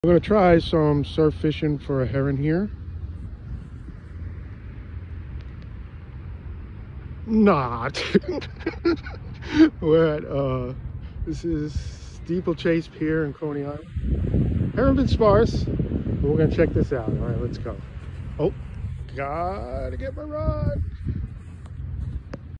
Herring Fishing at Coney Island Steeplechase Pier
Herring Fishing at Coney Island Steeplechase Pier - Getting going doing some herring fishing at Coney Island's own Steeplechase Pier